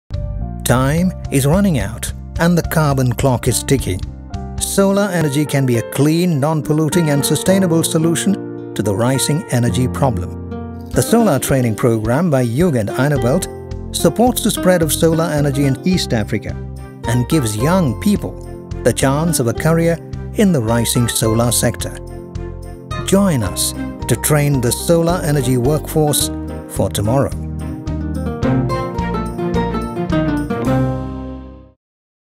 Solar Energy - Jugend Eine Welt Spot
Jugend Eine Welt and Don Bosco are working closely together in Ethiopia and Uganda to implement the ADA-sponsored solar programme in a sustainable way.

For more information on the Jugend Eine Welt Solar Programme, please